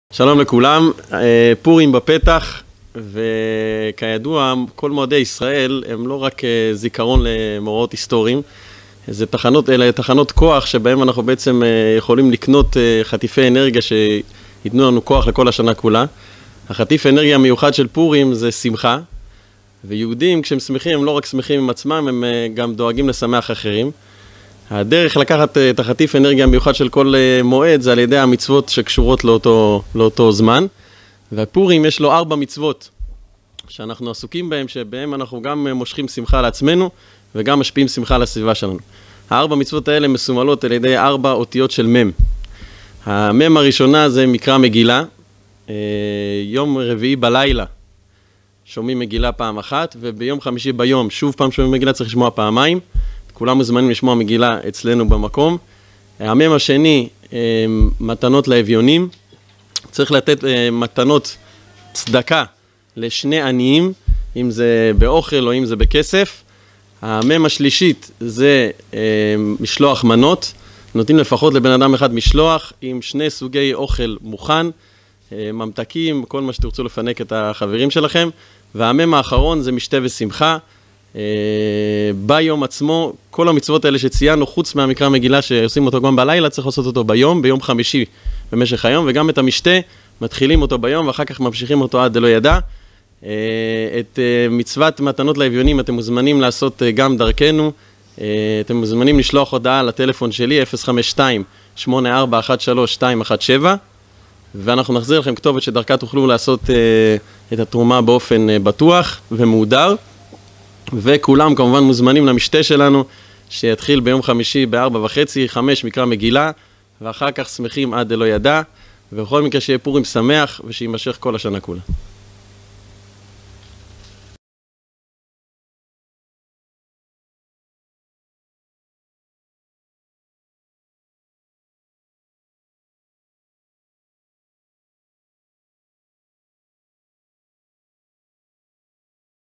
פורים שמח! ▪ ממעל ממש
מה עושים בפורים? תזכרו את ארבעת הממי"ם. הרב בן גור עם קטע קצר וקולע שיעשה לך סדר בחג השמח בשנה!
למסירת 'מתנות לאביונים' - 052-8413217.
מסיבת פורים ענקית ביום חמישי (24.4.16) החל מ-16:00 ועד שעות הלילה הקטנות...